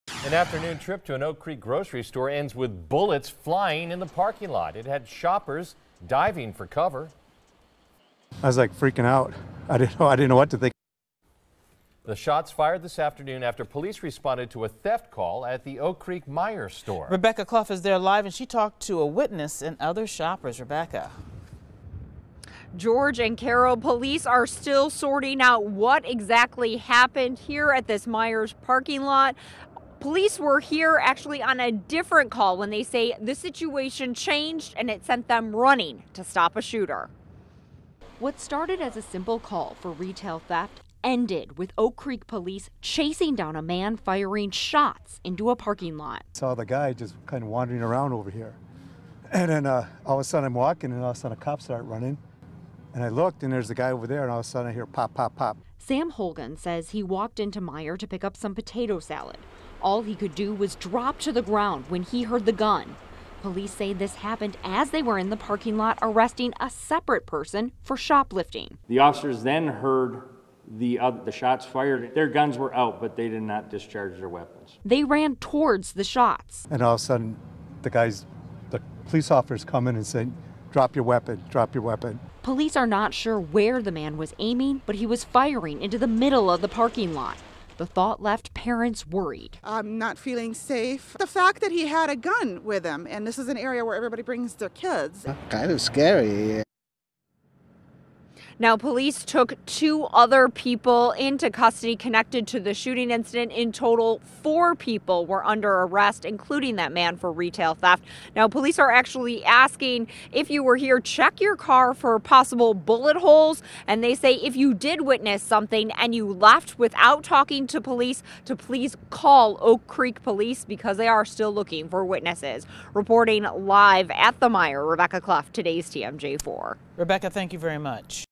Four in custody after gunshots fired near Oak Creek Meijer
Oak Creek police said four people are in custody after a theft and gunfire at the Meijer grocery store near Howell and Drexel.